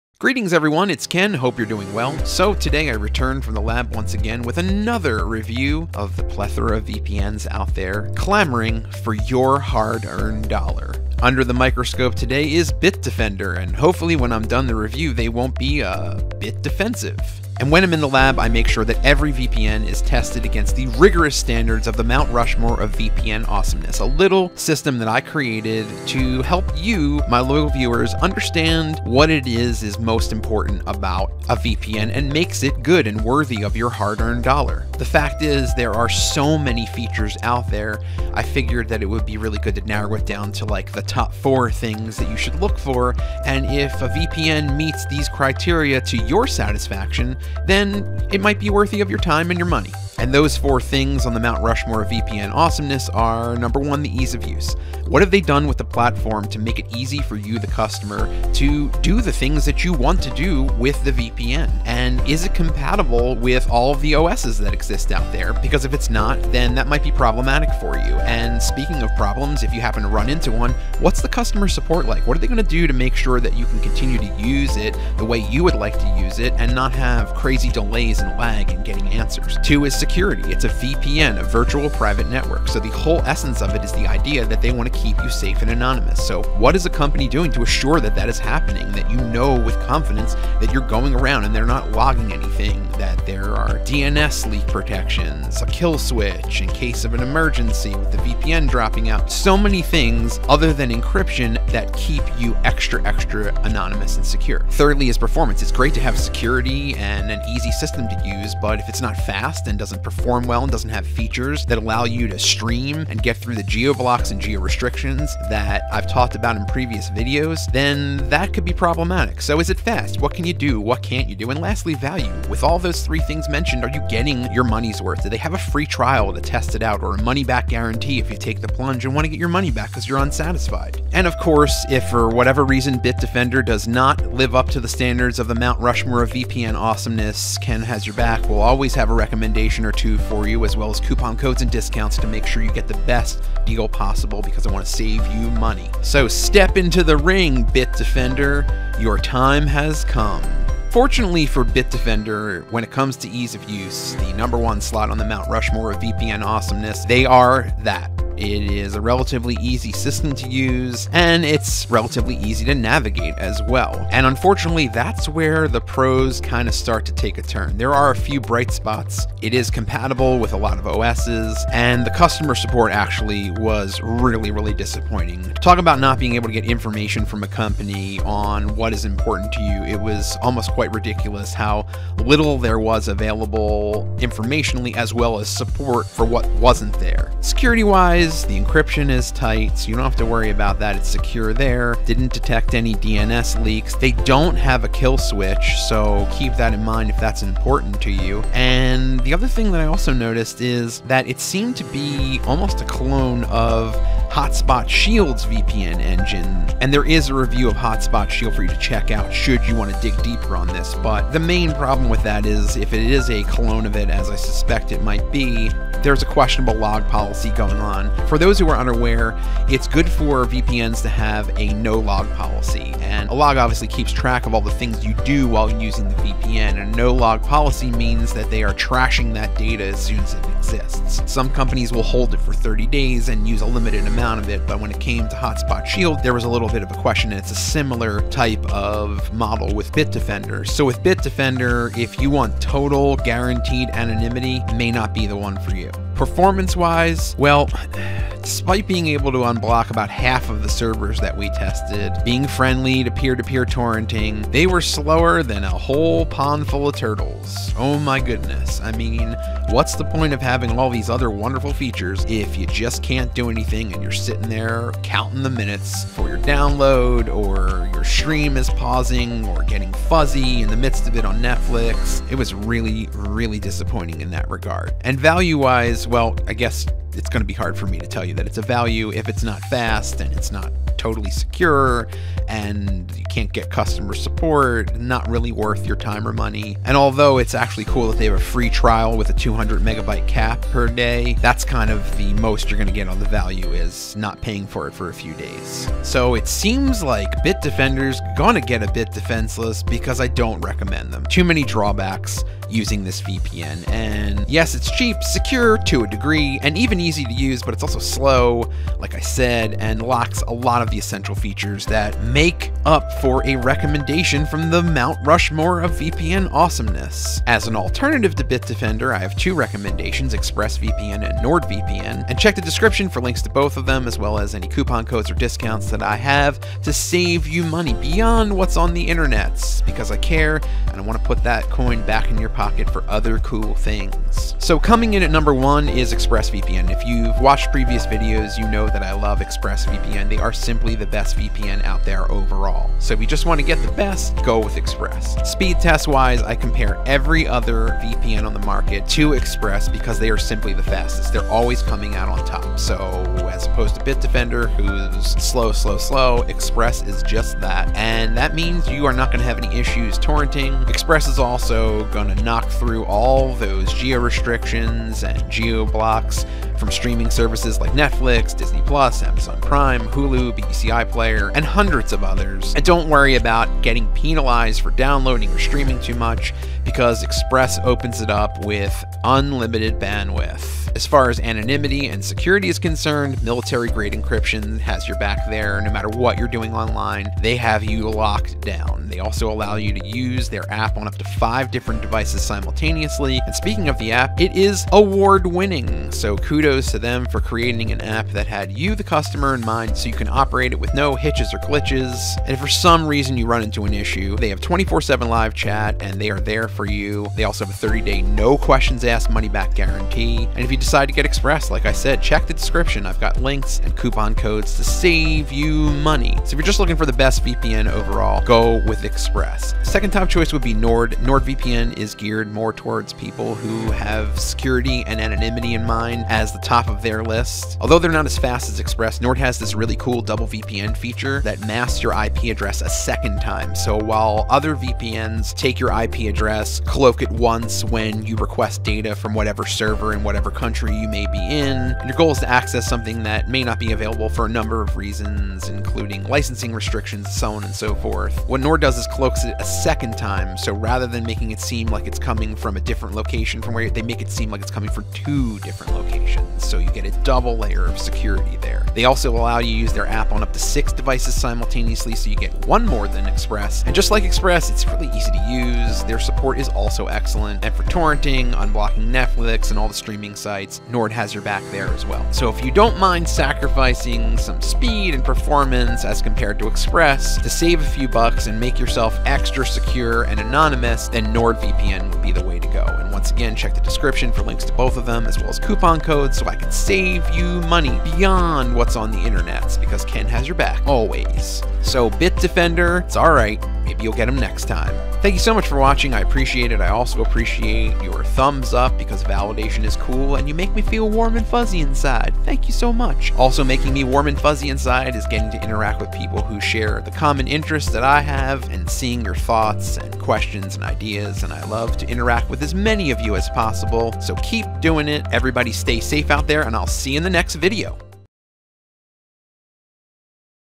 Bitdefender VPN Review of 2024 🔥100% BRUTALLY HONEST REVIEW!
🌐 Bitdefender VPN Review of 2024 🌐

Bitdefender VPN 2024 Review: Unveiling the Pros and Cons

Greetings, everyone! It's Ken, and today I've emerged from the lab with a comprehensive review of Bitdefender VPN. As always, I've subjected it to the rigorous standards of the Mount Rushmore of VPN awesomeness – a system I designed to help you, my loyal viewers, discern the crucial aspects that make a VPN truly worthwhile. Let's dive into the key elements:

Ease of Use and Compatibility

Bitdefender VPN claims its spot on the Mount Rushmore in terms of ease of use. Navigating through the system is relatively straightforward, ensuring a user-friendly experience. It aligns with a multitude of platforms, ensuring broader accessibility.

Security is the core essence of any VPN, and Bitdefender aims to keep users safe and anonymous. However, the review suggests a mixed bag. While encryption ensures a certain level of anonymity, the lack of detailed information on logging practices raises concerns. The absence of comprehensive DNS leak protection and an emergency kill switch adds ambiguity to the overall security framework.

In the realm of performance, Bitdefender exhibits a mixed bag of capabilities. It manages to provide a secure environment, but questions arise regarding speed and efficiency. The ability to navigate geo-blocks and restrictions, crucial for streaming and online activities, seems to be an area where Bitdefender may face challenges.

Lastly, the value proposition becomes pivotal in determining the overall worth of Bitdefender VPN. While it is relatively easy to use and offers compatibility with multiple systems, the question remains – is it delivering value for your money?

In conclusion, Bitdefender VPN, while excelling in ease of use and compatibility, encounters hurdles in customer support, security features, and performance. As always, I've got your back – if Bitdefender falls short of the Mount Rushmore standards, stay tuned for my recommendations and exclusive discounts to ensure you get the best VPN experience. Step into the ring, Bitdefender; the scrutiny of the Mount Rushmore awaits.

Hope you enjoyed my Bitdefender VPN Review of 2024 🔥100% BRUTALLY HONEST REVIEW! Video.